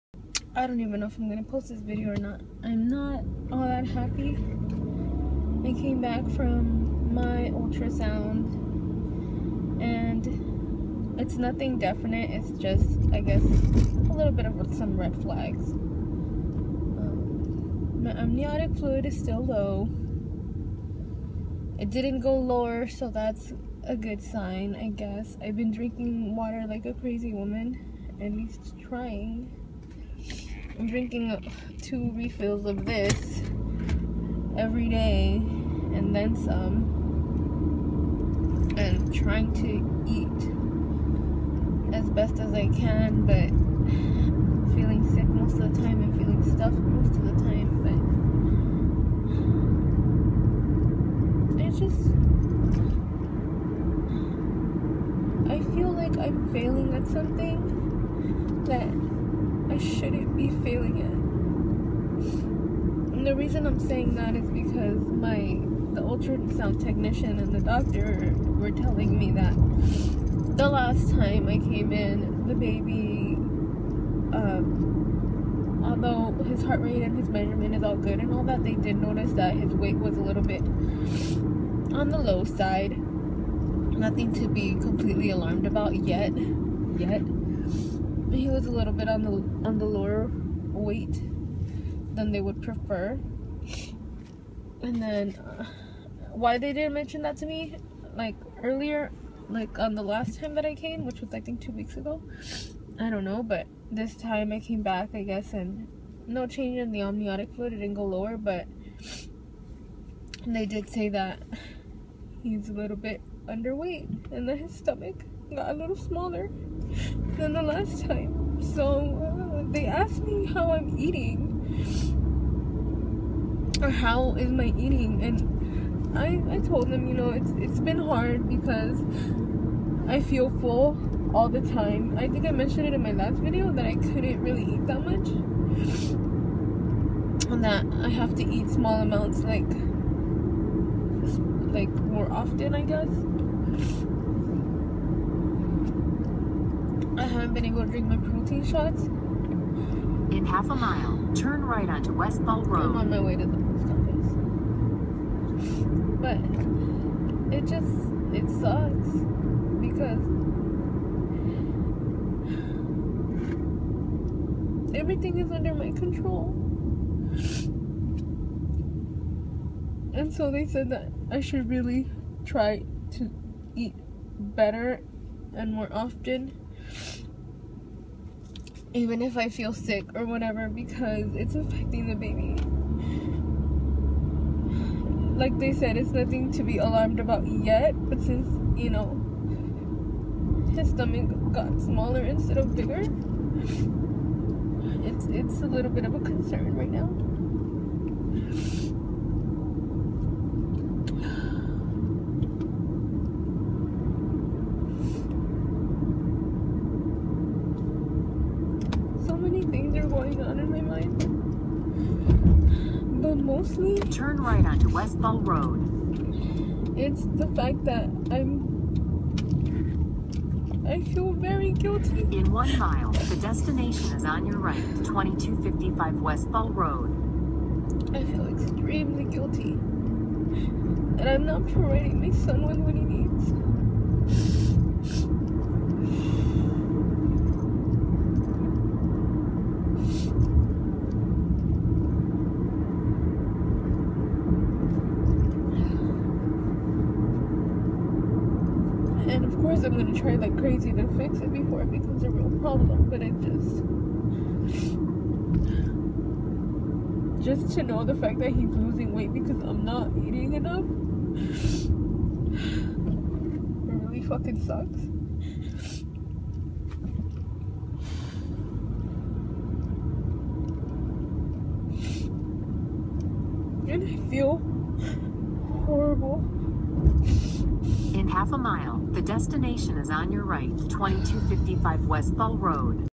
Sad and Scared
Hello Ladies and Gents!

Went to my ultrasound to check on the Amniotic Fluid and found that is has not gone up, but it has not gone down either...
The ultrasound tech informed me that although Artemis is weighing in at 4lbs, he is a bit small for 33 weeks. Also that his stomach has actually gotten smaller then the last ultrasound.  That if it does not increase in size in the next check up then it might be reason to be concerned.  I am so scared, sad, and most of all I feel guilty.
I have started to eat every hour to hour and a half , and started forcing myself to drink protein supplements...they make me feel so yucky.
IDK what else to do...besides try harder to eat. Wish me luck...